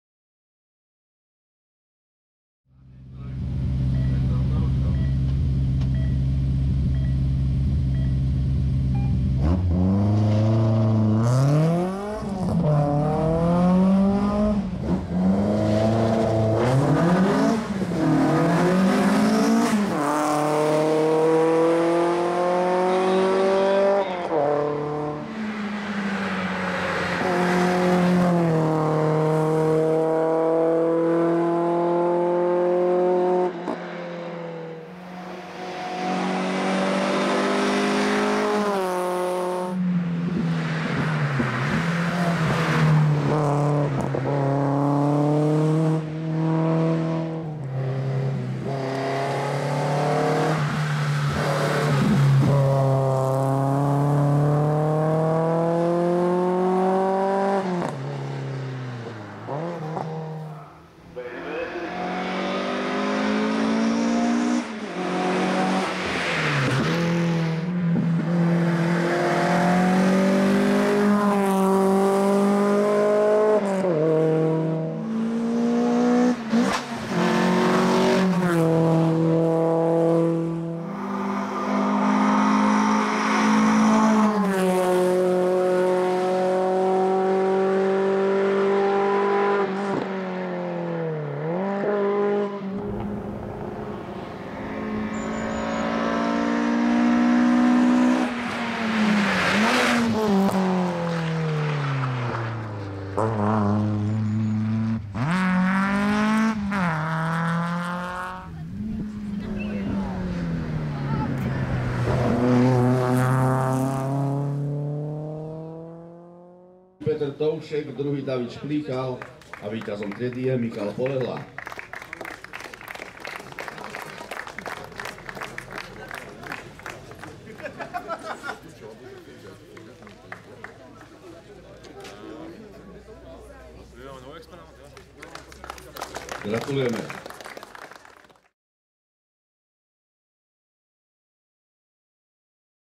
BMW E30 M3 - Petr Toušek - PRAMOS XVII. Divácká 13° - Diváky u Hustopečí 2022
Petr Toušek si s vozem BMW E30 M3 dojel pro třetí místo ve třídě HA1-2000 v závodech do vrchu PRAMOS XVII. Divácká 13° - Diváky u  Hustopečí 2022.